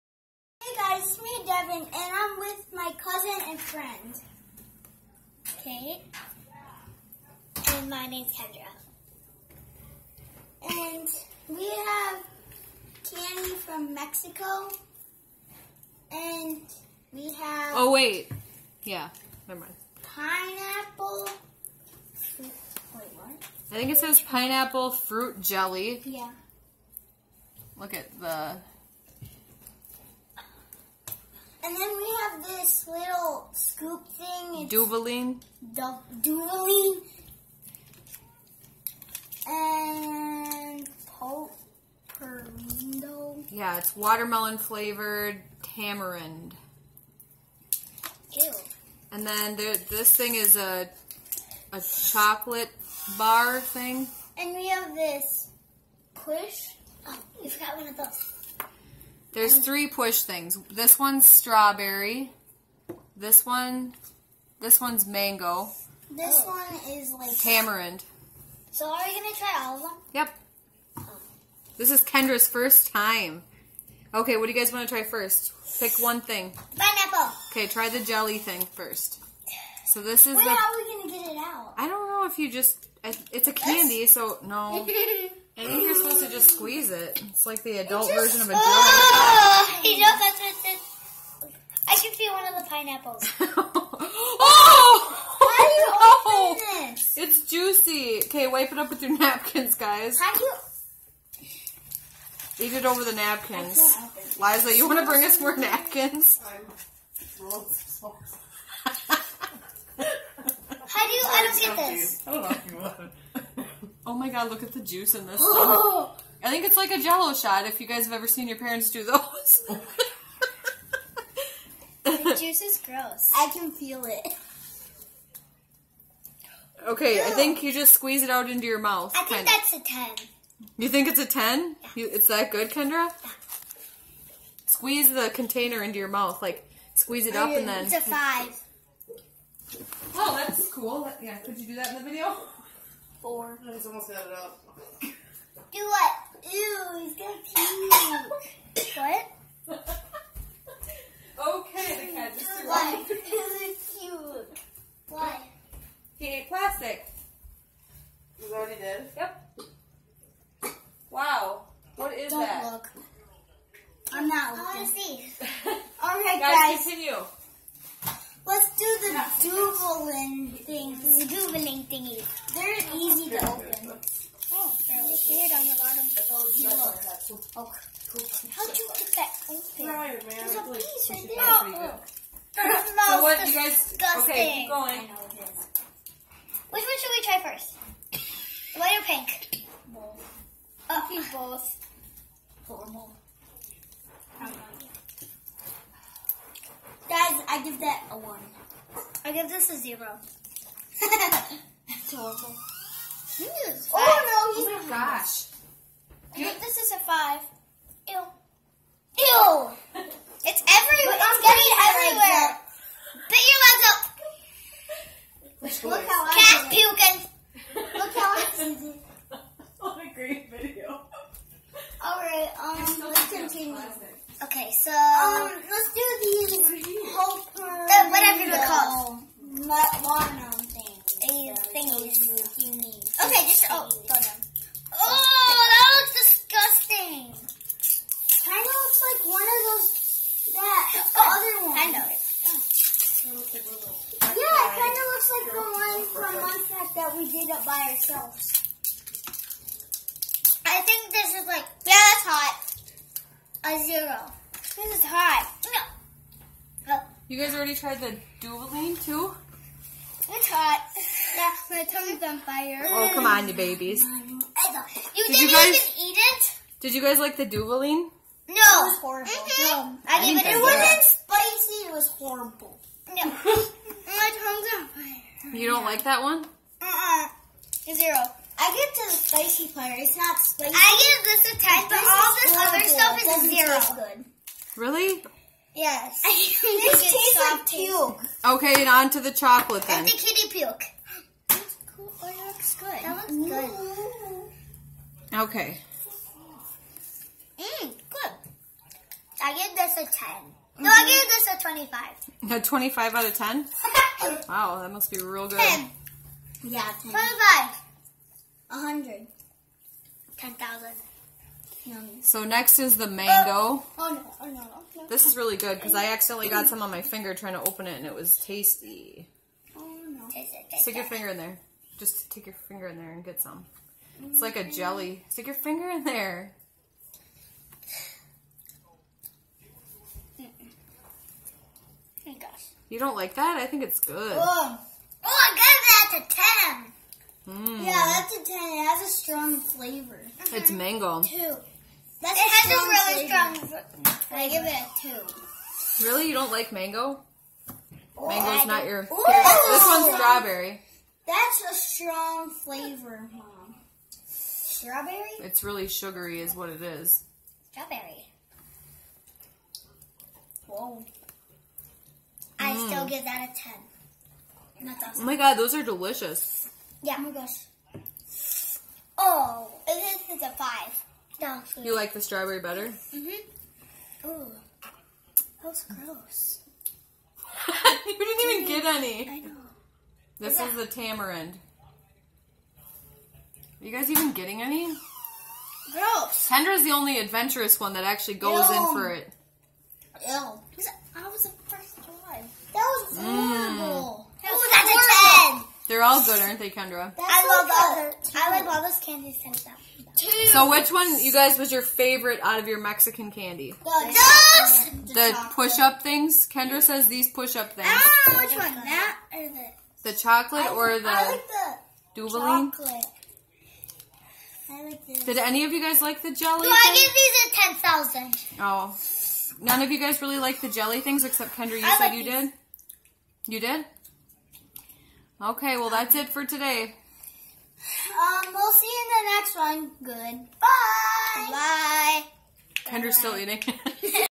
Me Kendra and Kate try Mexican candy
Tasting new candy we got from Mexico